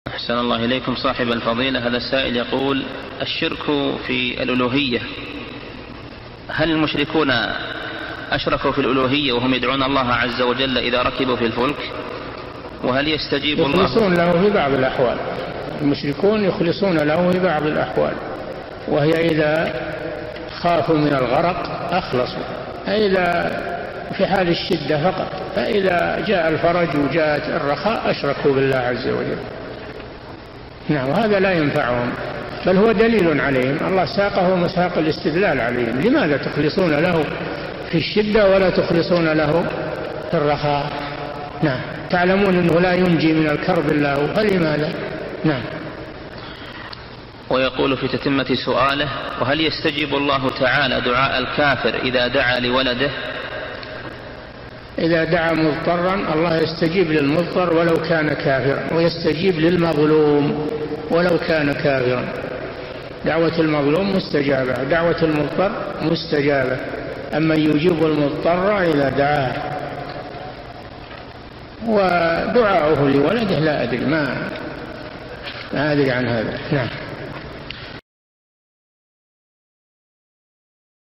[8\ 3436] الشرك في الألوهية واستجابة الله لدعاء الكافر | الشيخ صالح الفوزان
"السؤال رقم:8
الشرك في الألوهية، هل المشركون أشركوا في الألوهية وهم يدعون الله عز وجل إذا ركبوا في الفلك وهل يستجيب الله؟
الجواب:
المشركون يخلصون لله في بعض الأحوال، مثلما يفعلون إذا خافوا من الغرق في البحر، فيخلصون له في حالة الشدة فقط. أما إذا جاء الفرج والرخاء، أشركوا بالله عز وجل. هذا لا ينفعهم، بل هو دليلٌ عليهم؛ لأن الله سبحانه وتعالى يسوقهم مساق الاستدلال عليهم. لماذا تخلصون له في الشدة ولا تخلصون له في الرخاء؟ أنتم تعلمون أنه لا ينجي من الكرب إلا هو، فلماذا؟

ويقول في تتمة سؤاله: وهل يستجيب الله تعالى دعاء الكافر إذا دعا لولده؟
إذا دعا الكافر مضطراً، الله يستجيب للمضطر ولو كان كافراً، ويستجيب للمظلوم ولو كان كافراً. دعوة المظلوم مستجابة، ودعوة المضطر مستجابة، كما قال تعالى: {أَمَّنْ يُجِيبُ ‌الْمُضْطَرَّ إِذَا دَعَاهُ} [النمل: 62]. أما دعاؤه لولده، فلا أدري.

للاستفسار والإقتراحات والدعم
رئيس الفريق
لمشروع كبار العلماء